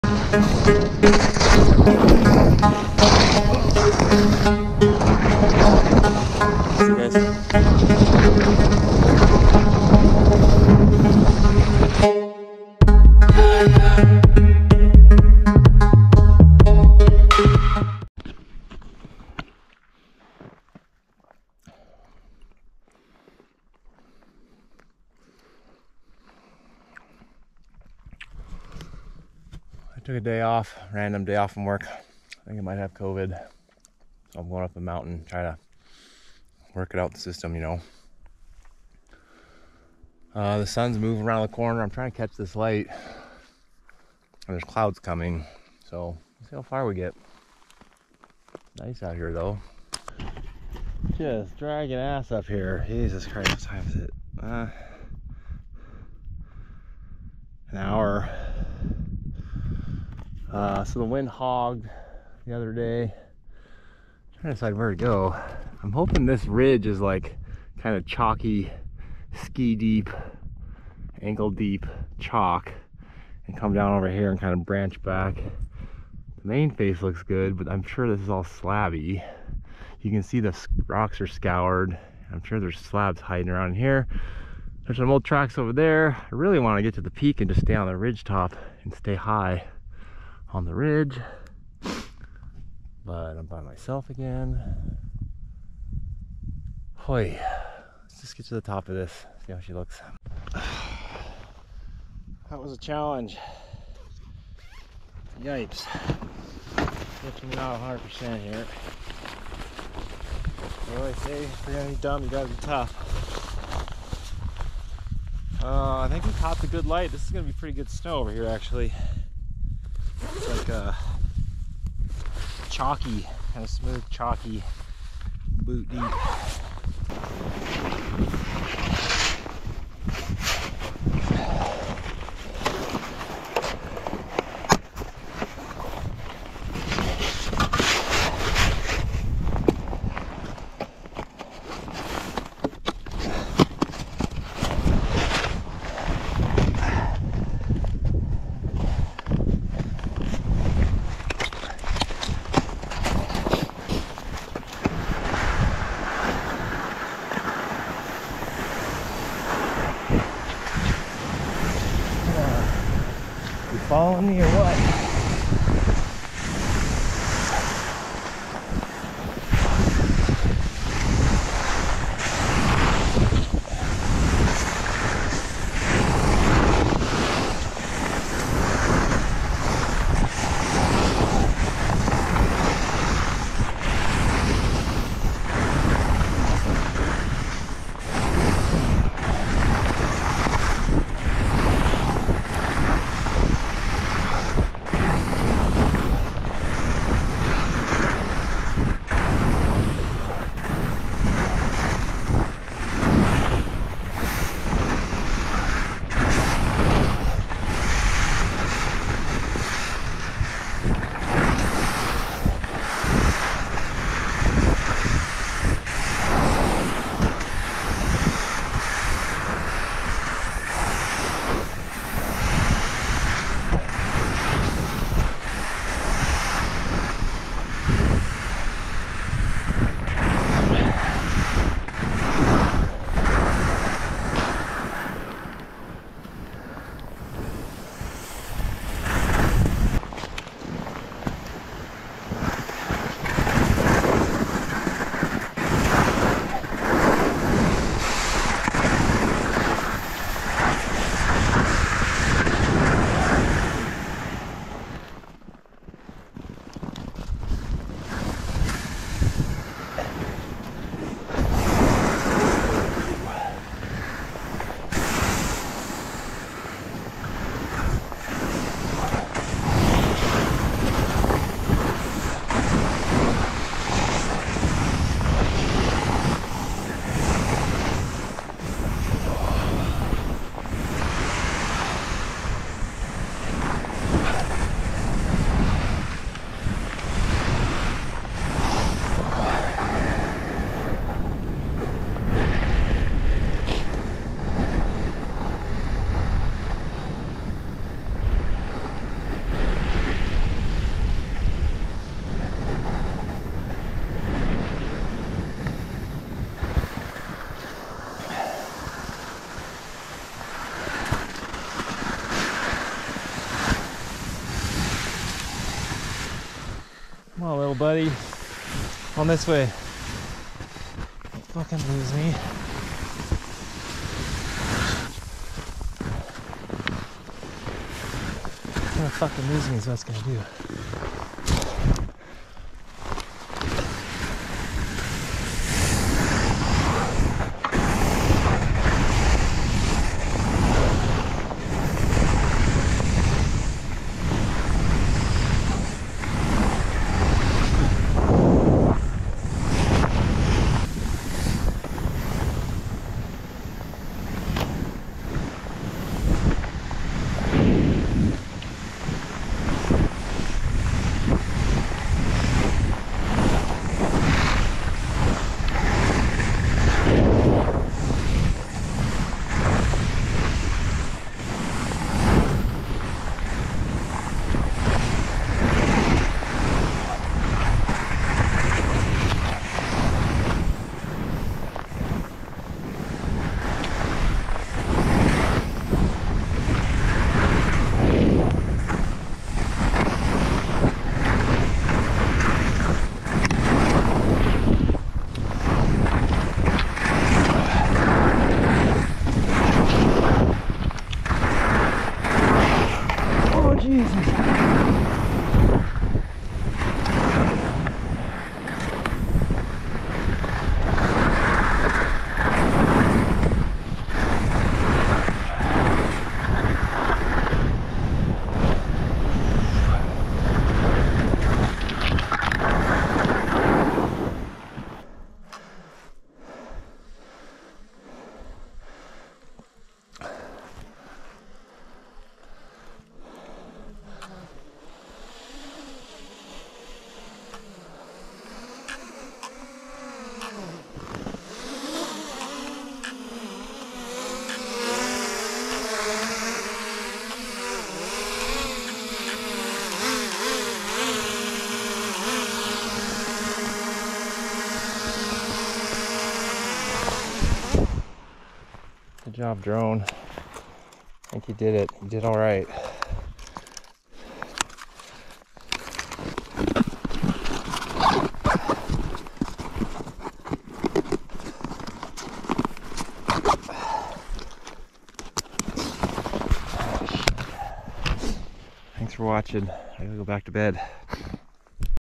Run of the Day: Ski Deep Chalk
Wind affected variable snow still made for some nice turns today!